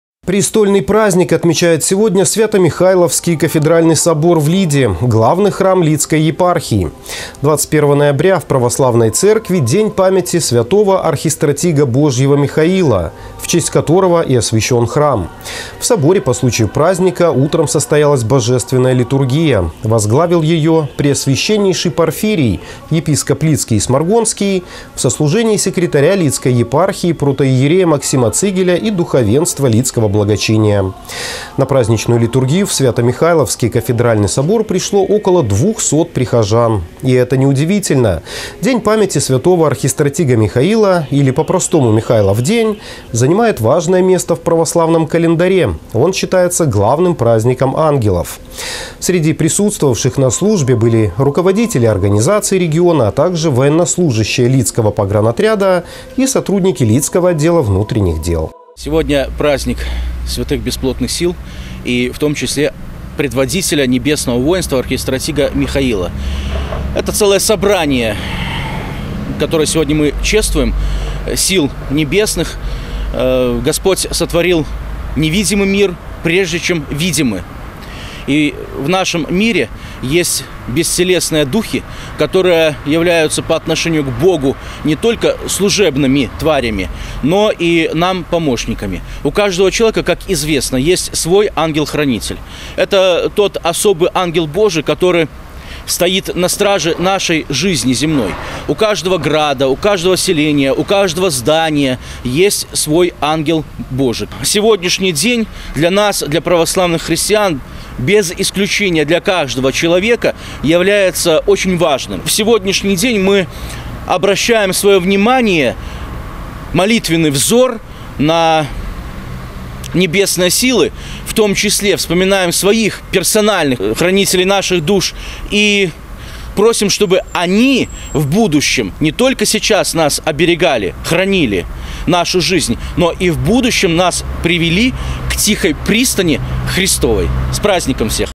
Престольный праздник отметил Свято Михайловский кафедральный собор
Престольный праздник отмечает сегодня Свято-Михайловский кафедральный собор в Лиде – главный храм Лидской епархии. 21-го ноября в православной церкви - День памяти святого Архистратига Божьего Михаила, в честь которого и освящен храм. В соборе по случаю праздника утром состоялась Божественная литургия. Возглавил ее Преосвященнейший Порфирий, епископ Лидский и Сморгонский в сослужении секретаря Лидской епархии протоиерея Максима Цигеля и духовенства Лидского благочиния. На праздничную литургию в Свято-Михайловский кафедральный собор пришло около двухсот прихожан. И это не удивительно: День памяти святого Архистратига Михаила, или по-простому Михайлов день, занимает важное место в православном календаре: он считается главным праздником ангелов. Среди присутствовавших на службе были руководители организаций региона, а также военнослужащие Лидского погранотряда и сотрудники Лидского отдела внутренних дел.